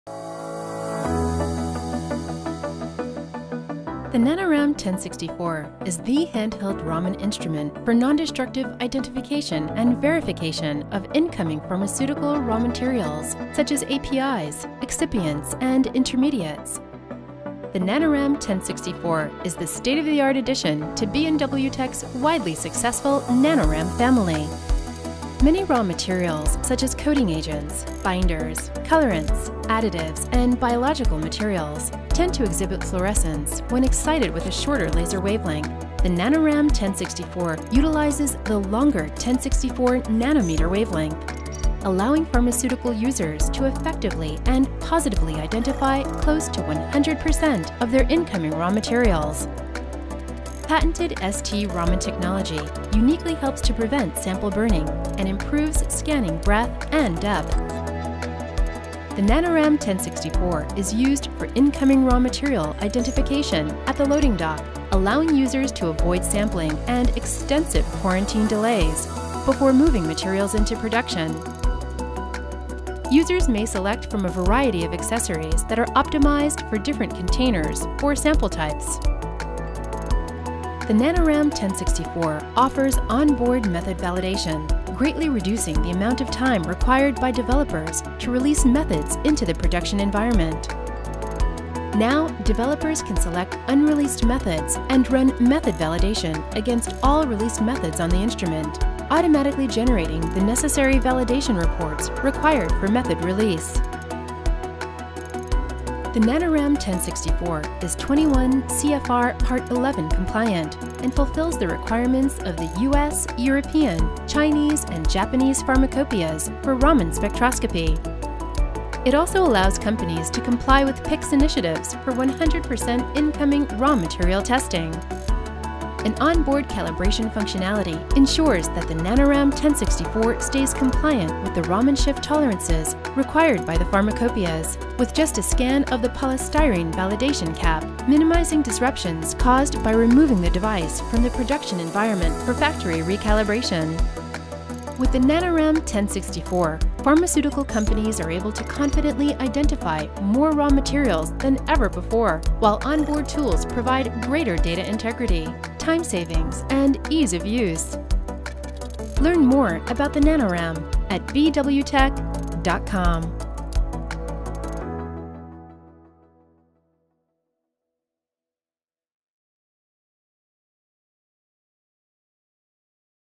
The NanoRam-1064 Handheld Raman Analyzer from B&W Tek
The NanoRam-1064 handheld Raman spectrometer from B&W Tek is the newest raw material identification device available to pharmaceutical companies looking to conduct 100% incoming raw material identification. The NanoRam-1064 employs the 1064nm laser, reducing fluorescence interference and allowing users to successfully identify materials in the pharmaceutical chain.